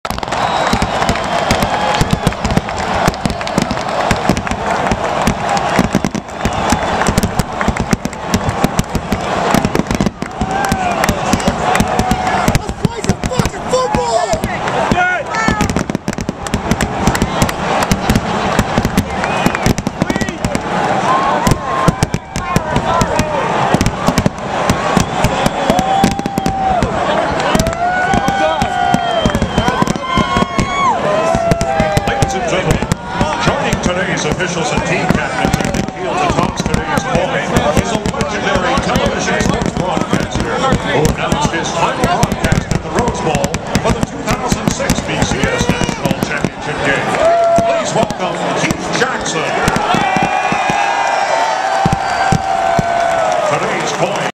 Pre-game Fireworks: 2010 BCS National Championship
The pre-game fireworks over the Rose Bowl in Pasadena, CA, prior to the BCS National Championship game between the Texas Longhorns and the Alabama Crimson Tide.